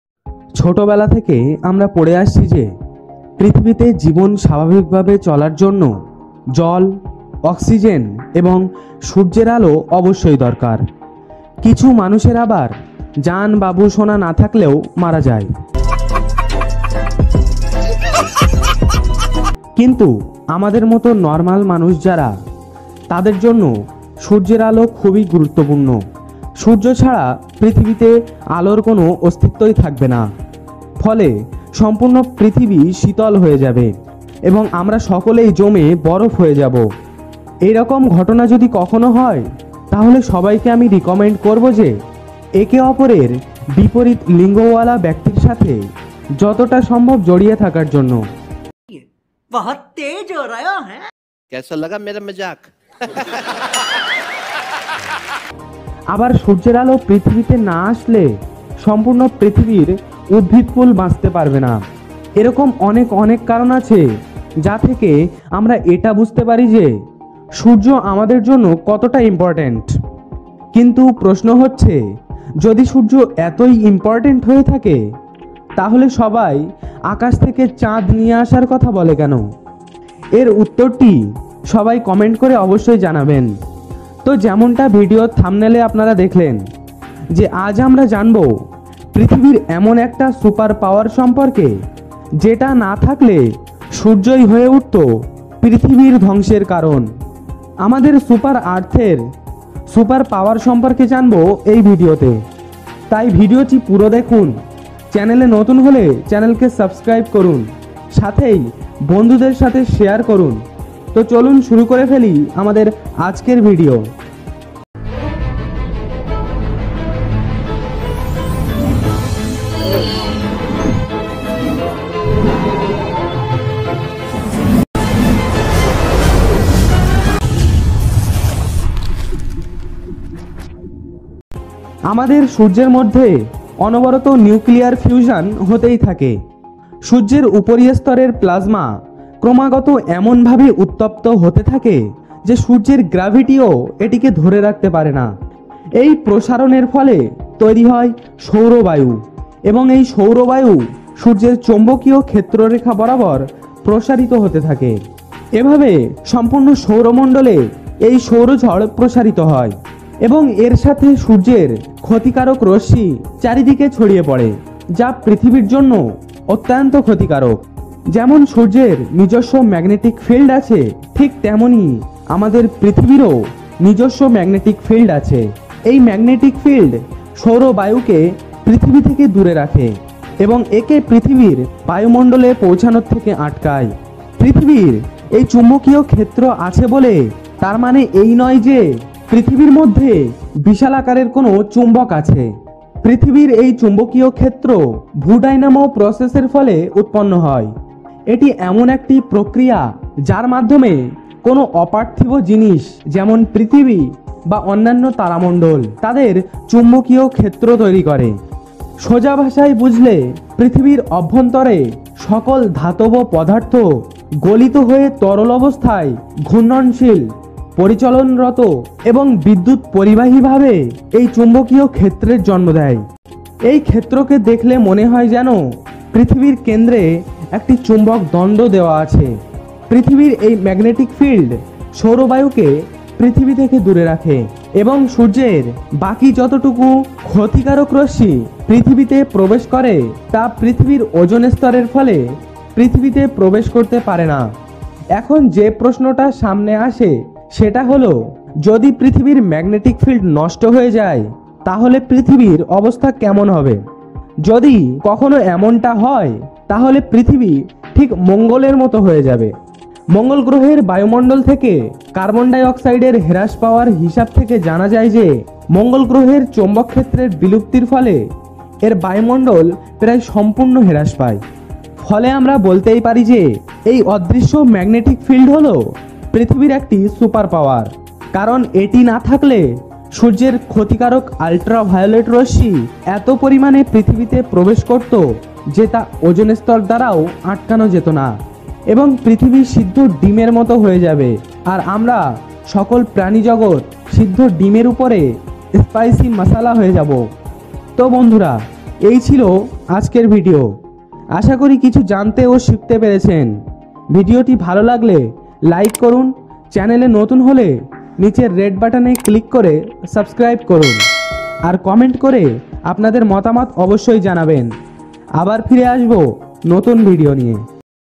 পৃথিবীর কাছে আছে এক অলৌকিক শক্তি । Earth have a god gifted super power
জানব পৃথিবীর এমন একটা super power সম্পর্কে যেটা না থাকলে সূর্যই হয়ে উঠত পৃথিবীর ধ্বংসের কারণ । আমাদের super earth এর super power সম্পর্কে জানব এই ভিডিও তে ।তাই ভিডিও টি পুরো দেখুন ।
Original Content Created By Rohossodut

Script          : Rohossodut(Admin)
Audio          : Rohossodut(Admin)
Raw Materials : Internet
Editing          : Rohossodut(Admin)